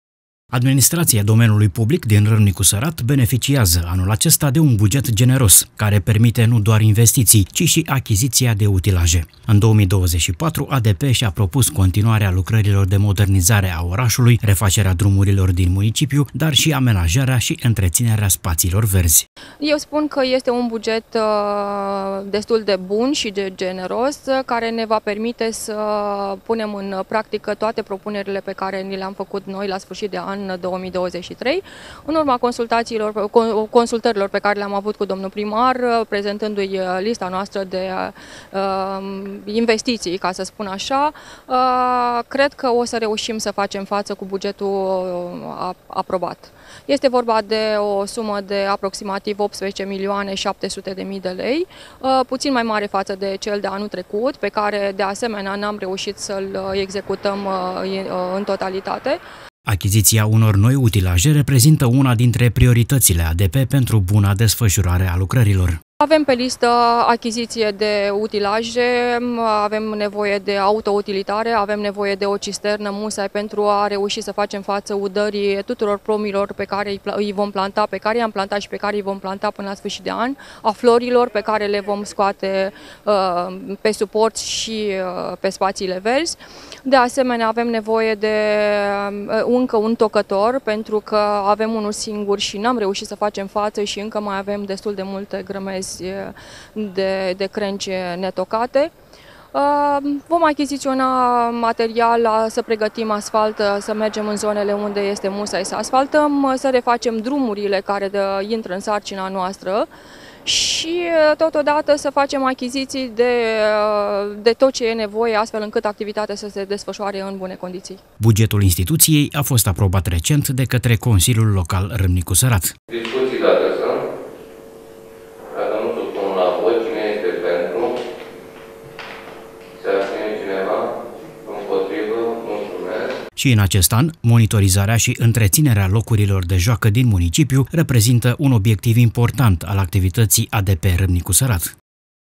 UN BUGET GENEROS
Administrația Domeniului Public din Râmnicu Sărat beneficiază, anul acesta, de un buget generos, care permite nu doar noi investiții, ci și achiziția de utilaje